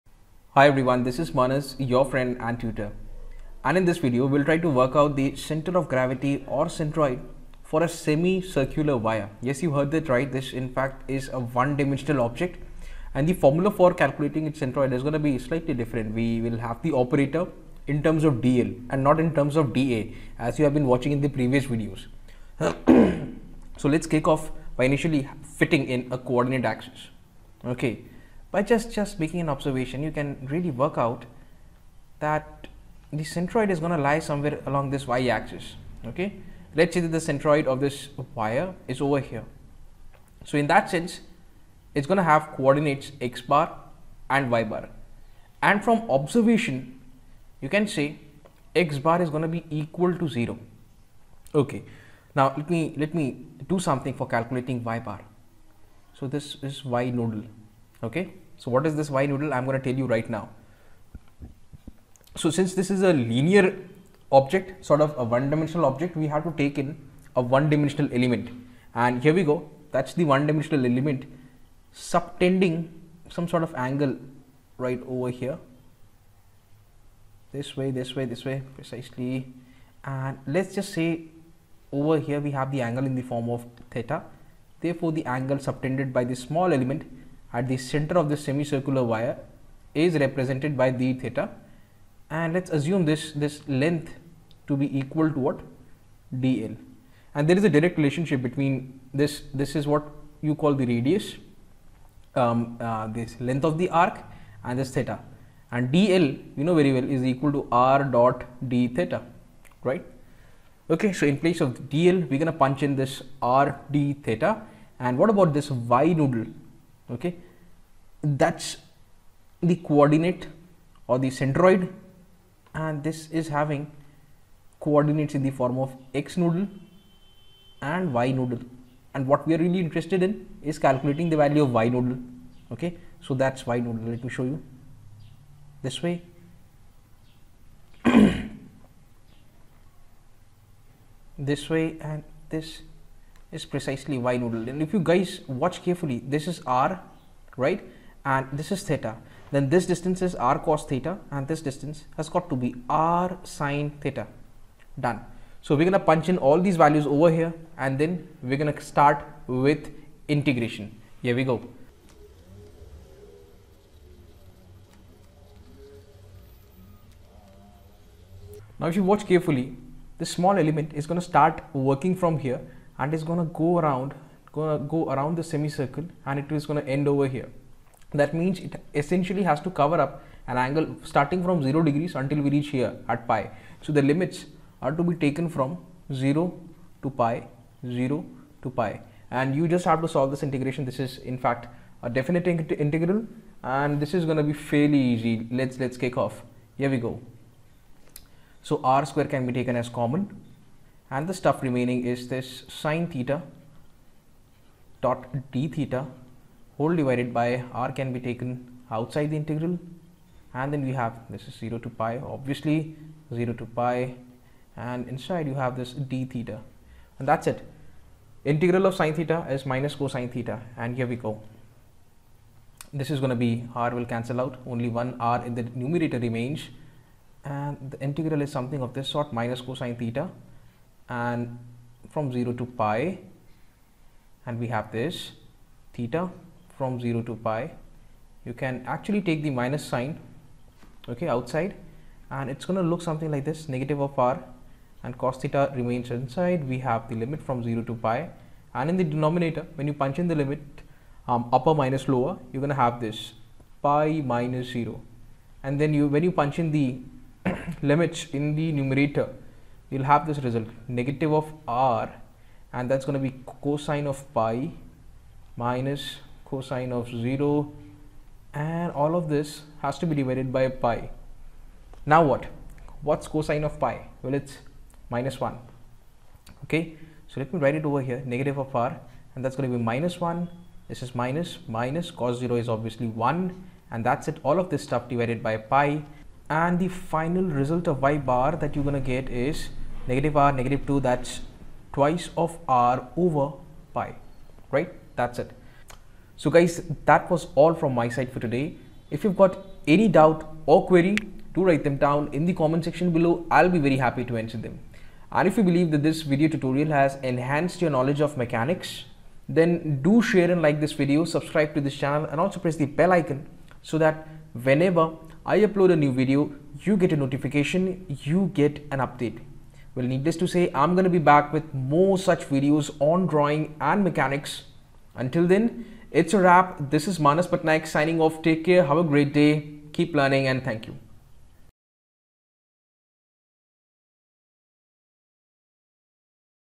How to find Centroid of a Semi Circular Wire by Integration
Hi Everyone...
In this video we will find the centroid/center of gravity of a SemiCircularwire by Integration.